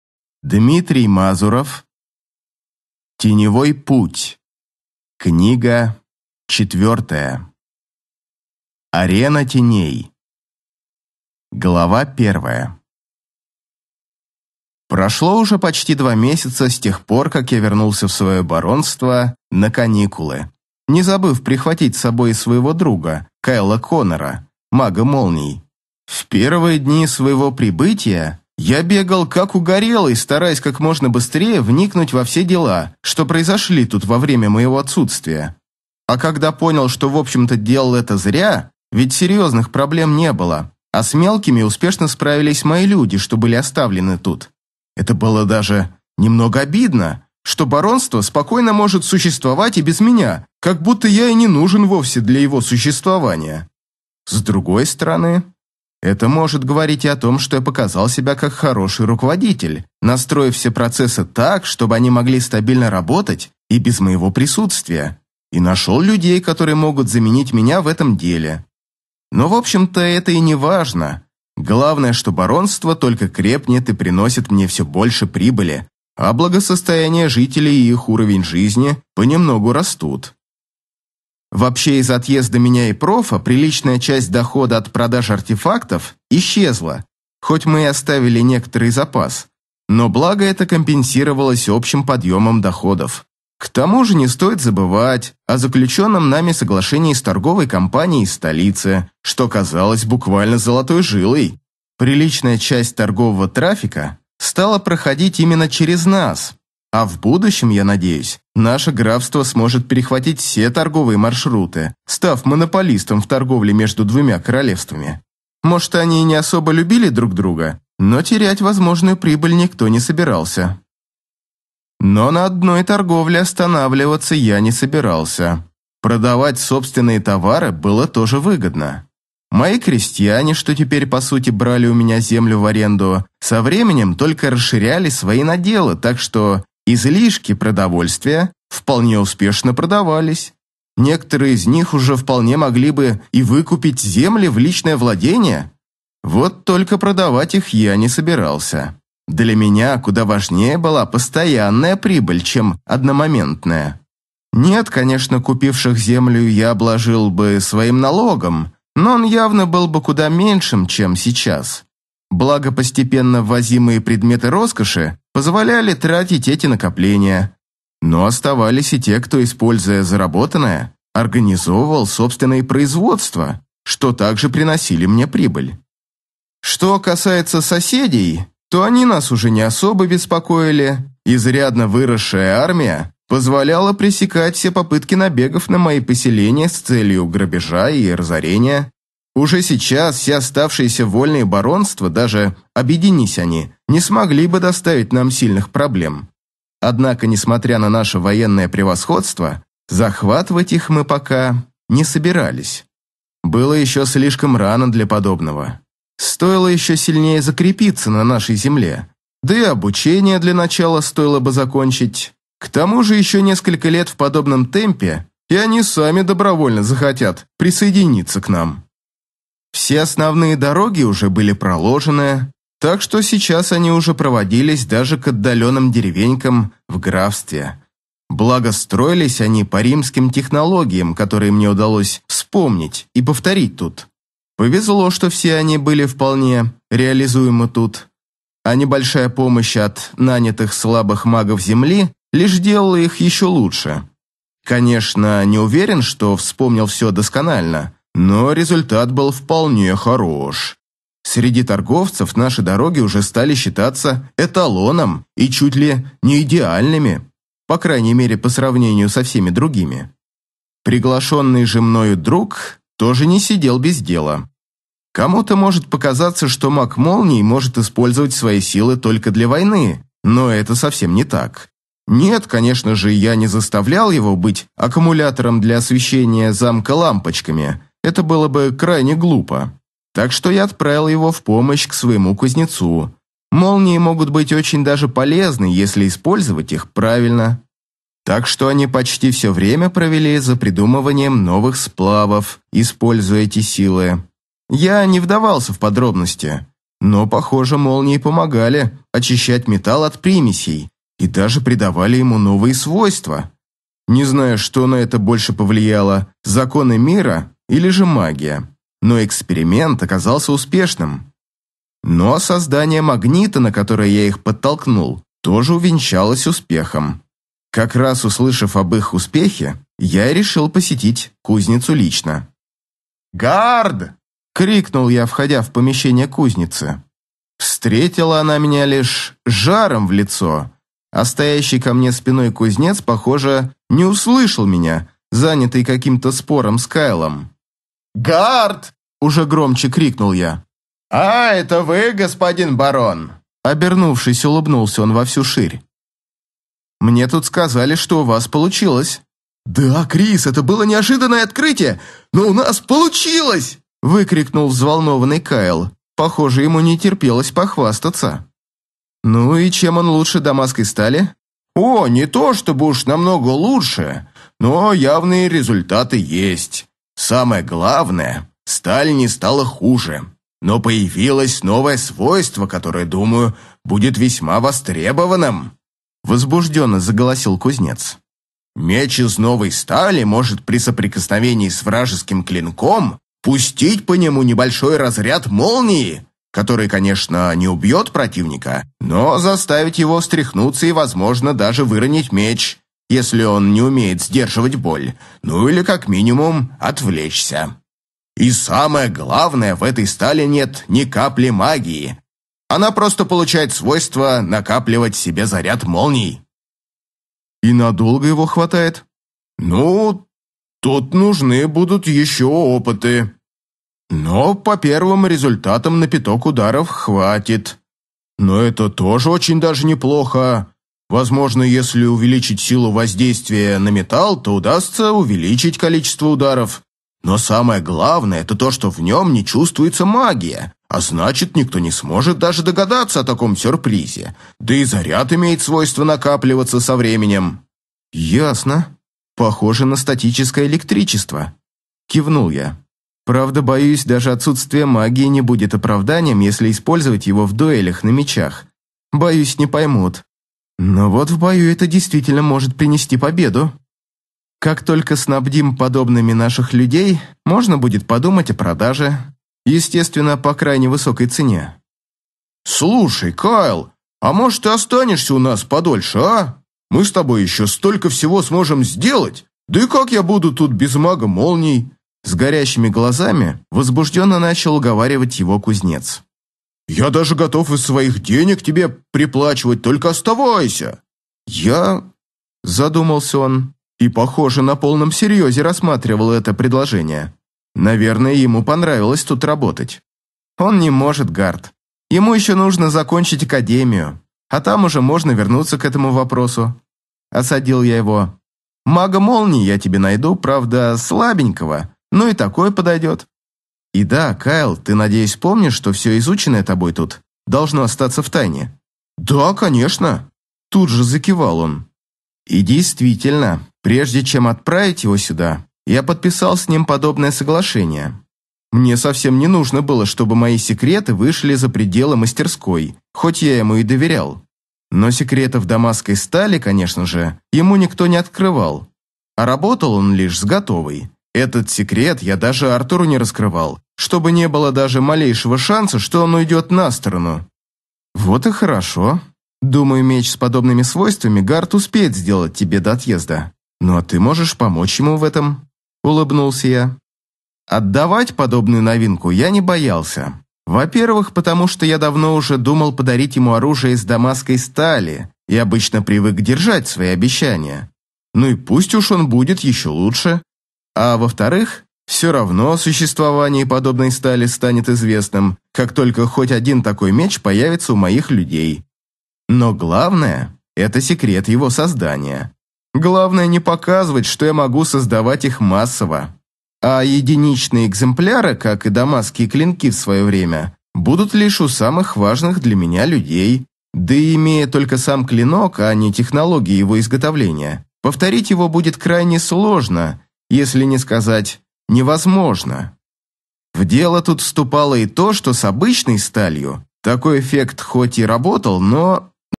Арена теней | Дмитрий Мазуров (аудиокнига)
Прослушать и скачать полностью аудиокнигу «Теневой путь | Дмитрий Мазуров» можно на сайте
Описание книги

Крис Дарк – маг теней, бывший убийца продолжает обучение в академии магии. В этот раз ему предстоит раскрыть тайны, которые он не должен был знать. Сразиться сразу в двух турнирах. Сможет ли он победить и какие ещё преграды ждут его на пути?

    Чтец: Михаил Алексеев
    Дизайнер обложки: Анастасия Церковская
    Правообладатель: Автор